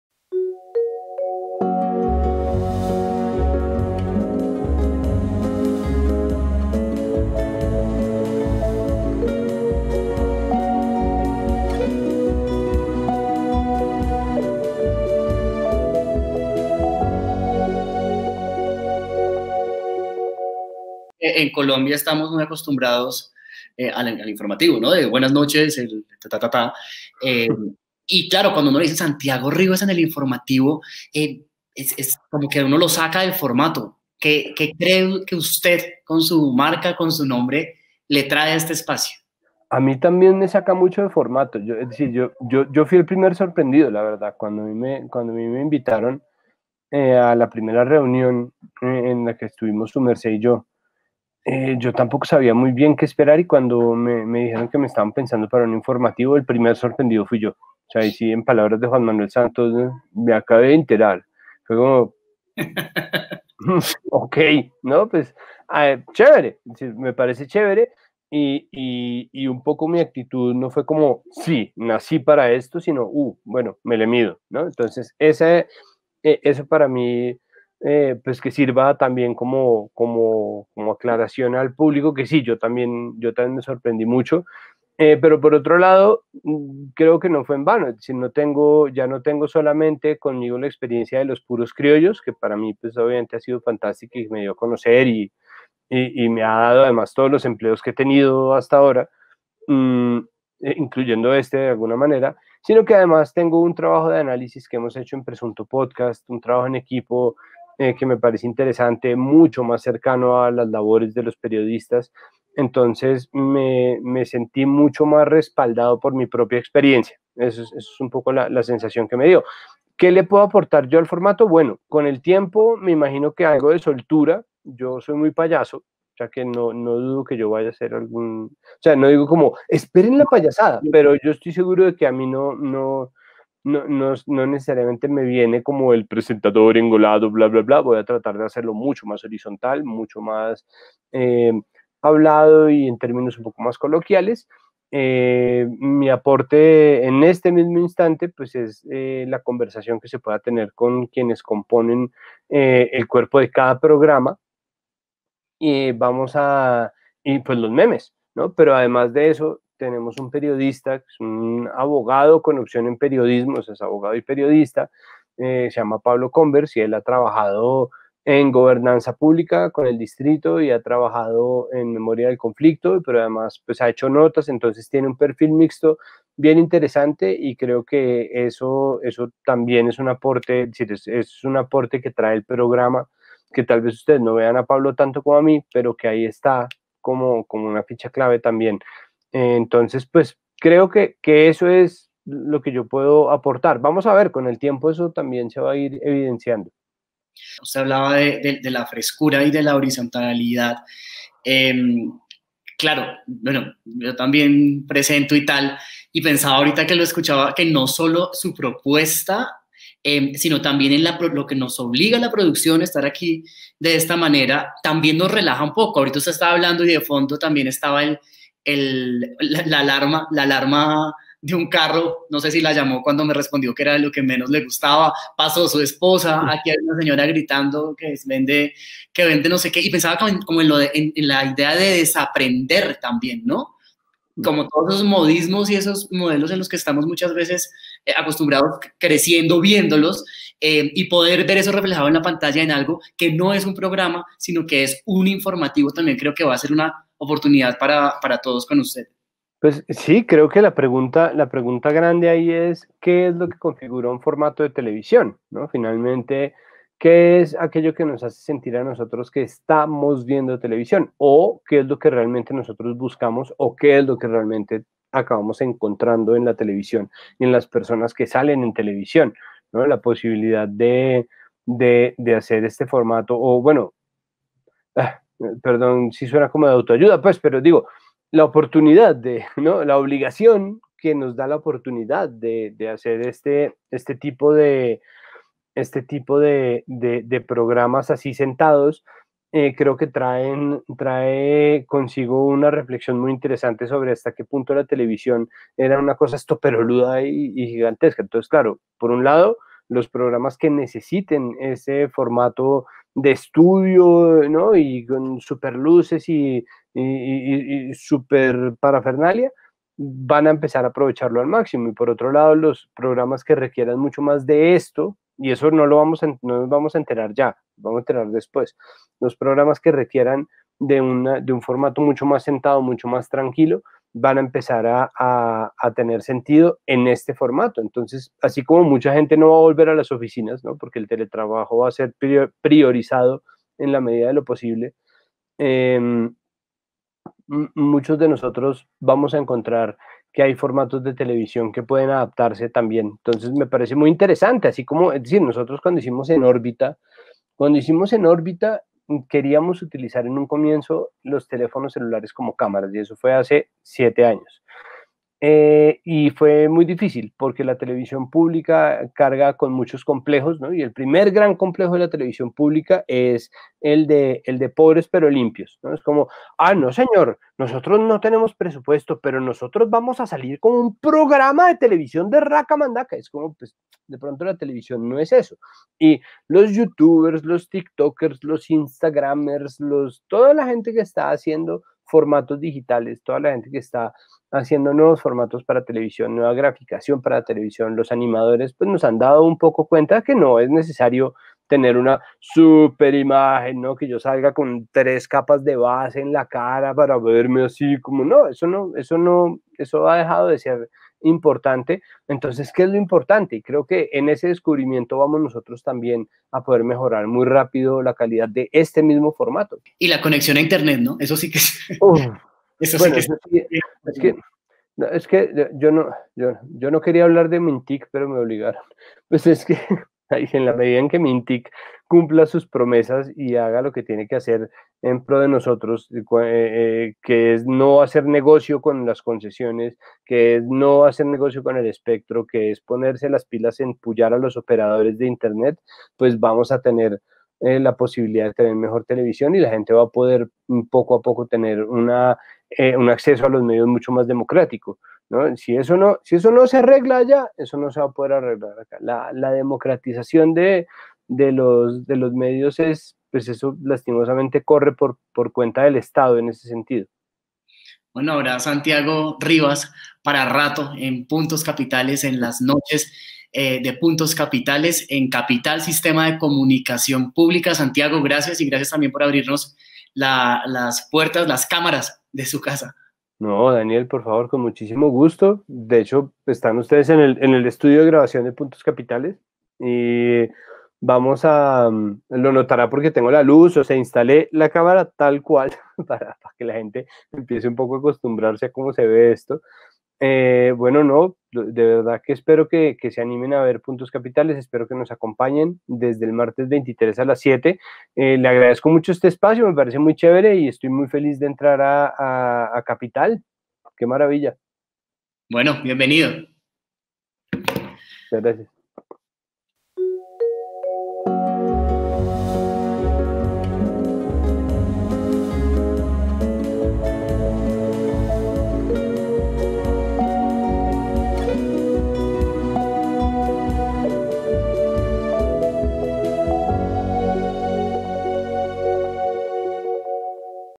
Santiago Rivas llega a Capital con Puntos Capitales - Entrevista - Parte 3
Esta es la tercera y última parte de la entrevista de Daniel Téllez a nuestro nuevo conductor: Santiago Rivas, quien estará a la cabeza de nuestro nuevo programa PuntosCapitales.

"Así como mucha gente no volverá a las oficinas, porque el teletrabajo va a ser priorizado, muchos de nosotros vamos a encontrar que hay formatos de televisión que pueden adaptarse", afirma.